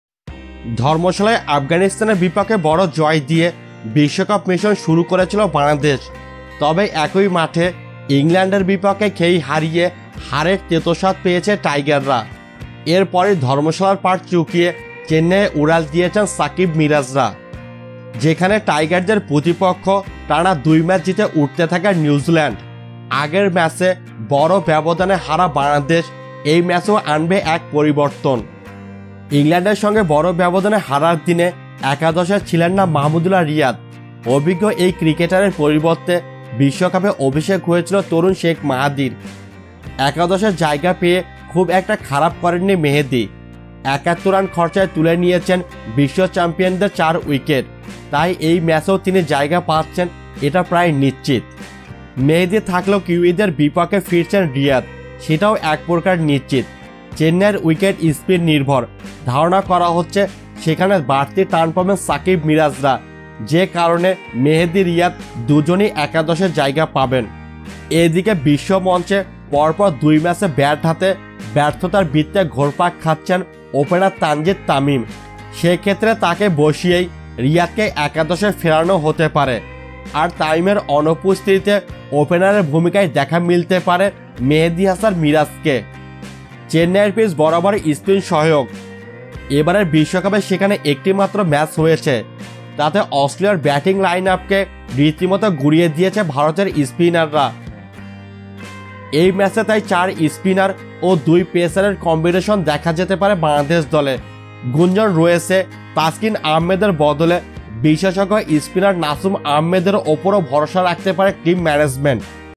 ফিরছেন মাহমুদউল্লাহ, একাদশে থাকবে শেখ মাহেদী তাহলে বাদ পড়বে কে?
Bangladesh vs New Zealand || Ban vs NZ || Sports 24 || Mahmudullah Riyad || Tanzid Hasan Tamim || Nazmul Hossain Shanto || Bangladesh Playing 11 || ICC World Cup